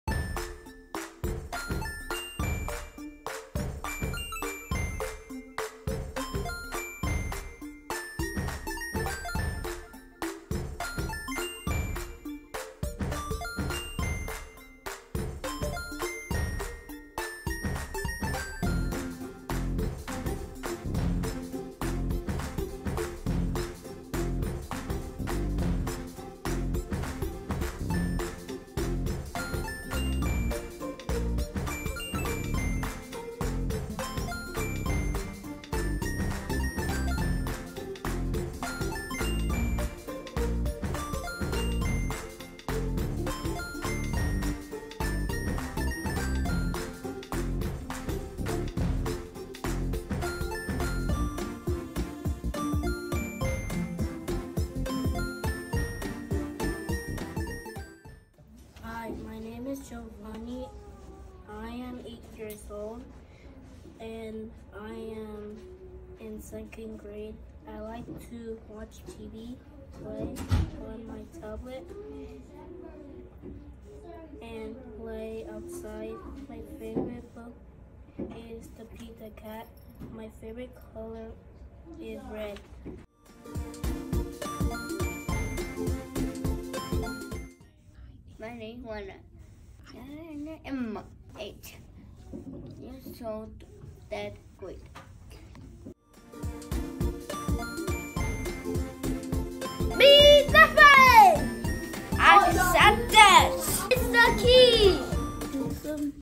Autism Acceptance Month - April 25, 2024 (ver. 2)
Cherokee Lane Elementary School
Autism Acceptance Month Morning Announcements